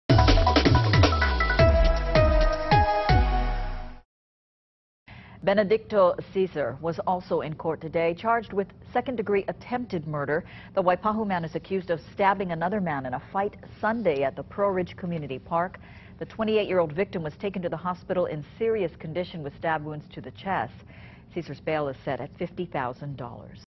Waipahu man faces charges of attempted murder
Benedicto Ceasar appeared in court Wednesday to face charges of second degree attempted murder.